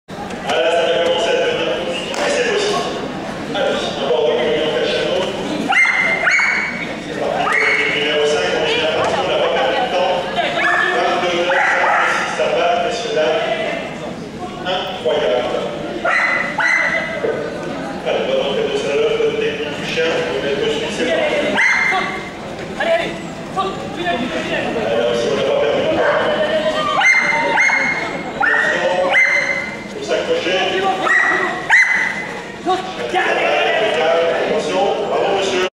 Guimauve Jumping plus Le Creusot novembre 2015
Concours d'agility, Le Creusot 15 novembre 2015, Jumping "plus" catégorie C